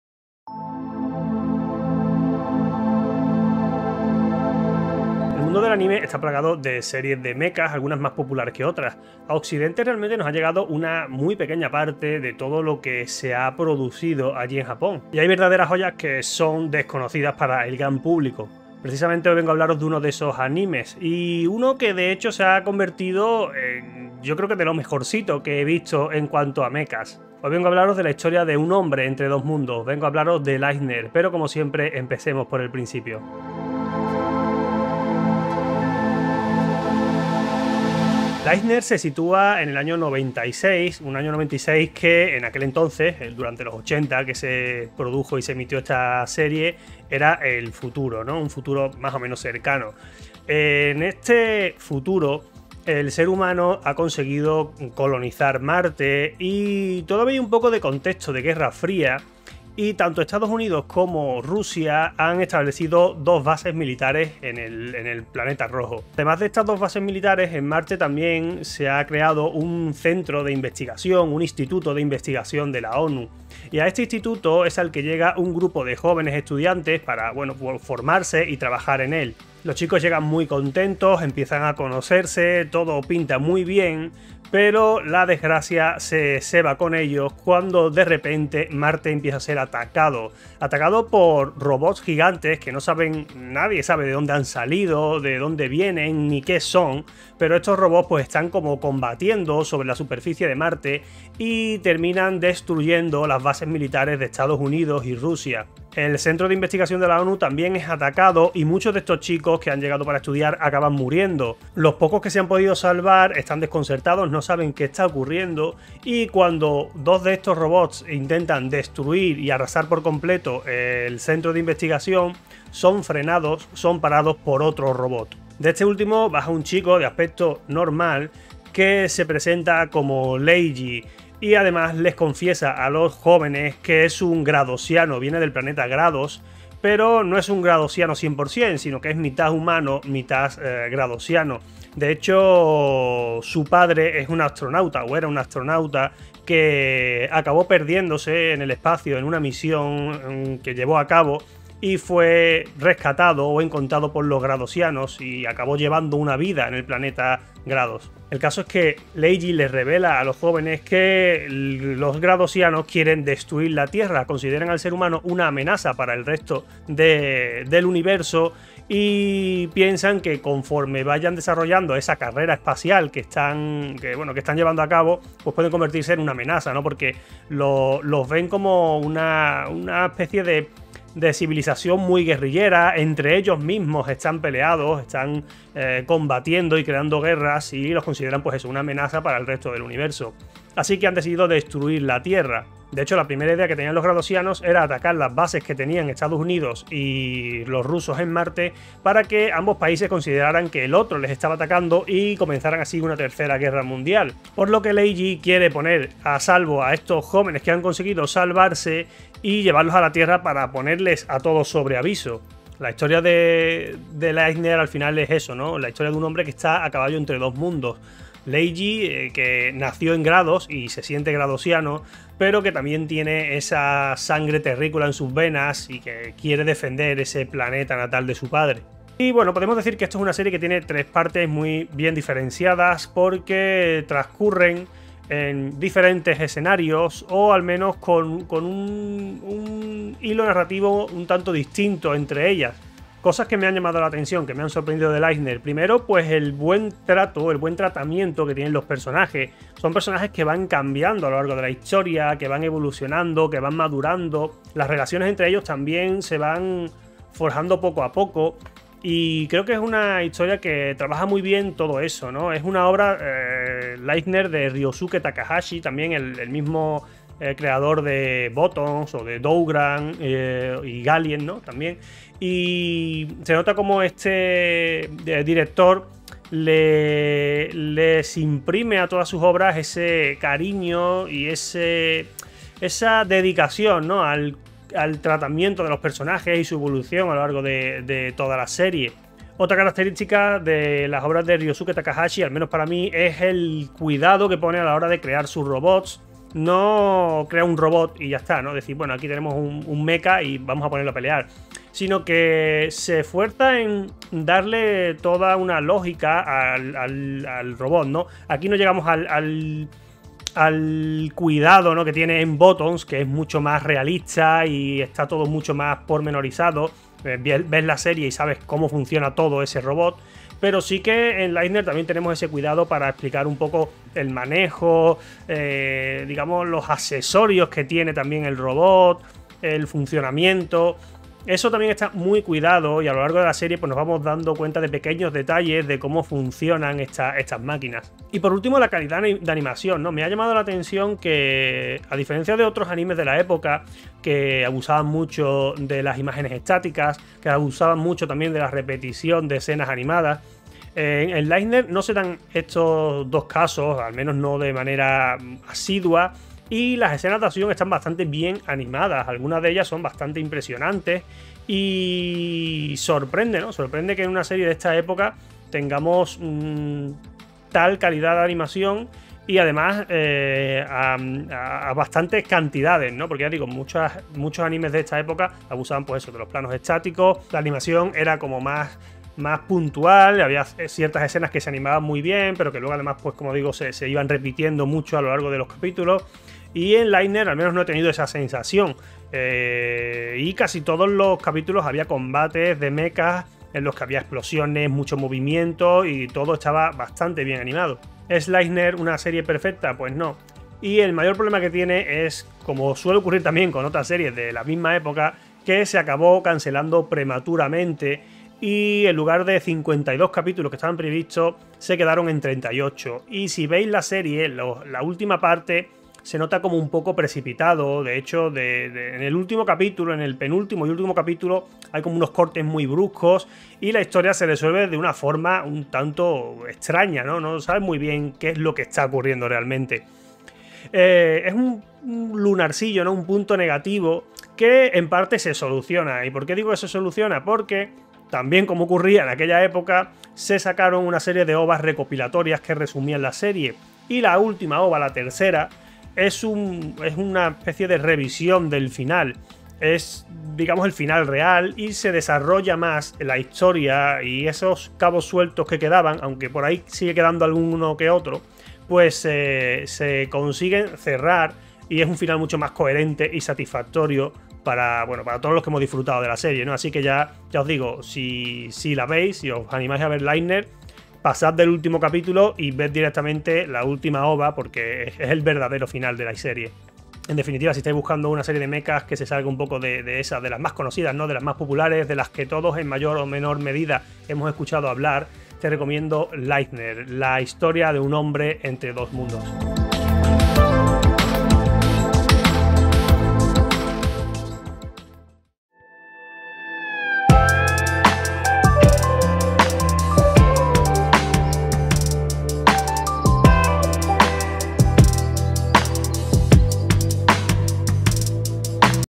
Un hombre entre dos mundos - Blue Comet SPT Laizner
Recomendación dela serie anime de mechas Laizner,
🛒Compra tus mangas en Amazon y apoya el canal